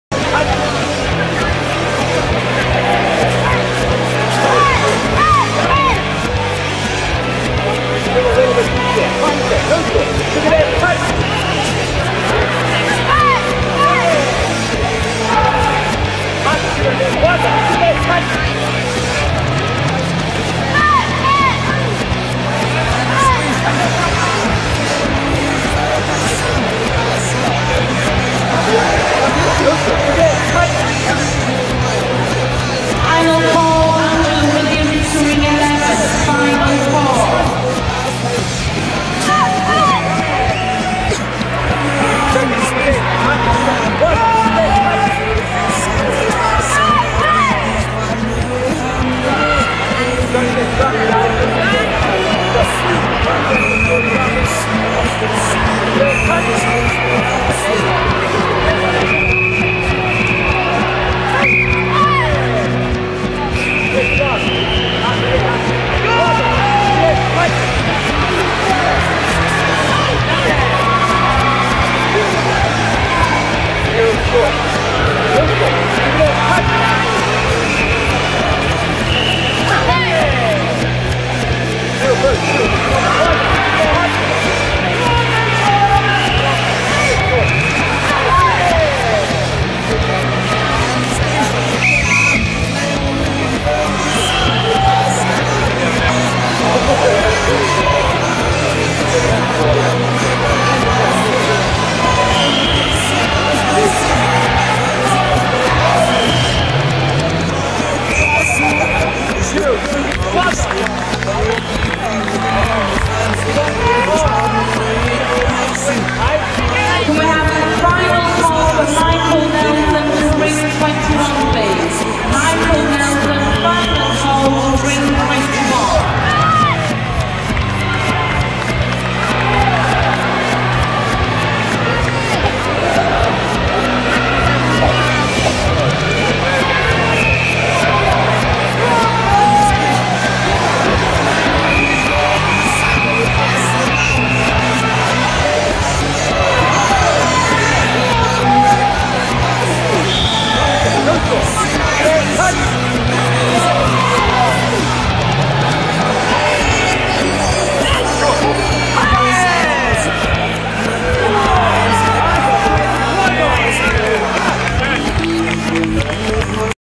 gkr wc4 2007 06 02 male female 3rd+ kumite 07
GKR Karate
World Cup 4
Birmingham, England
Arena Birmingham (NIA - National Indoor Arena)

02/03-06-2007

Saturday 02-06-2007

Male & Female 3rd kyu+ Kata & Kumite

Photos can be found on Flickr here for Pre and Post Event Photos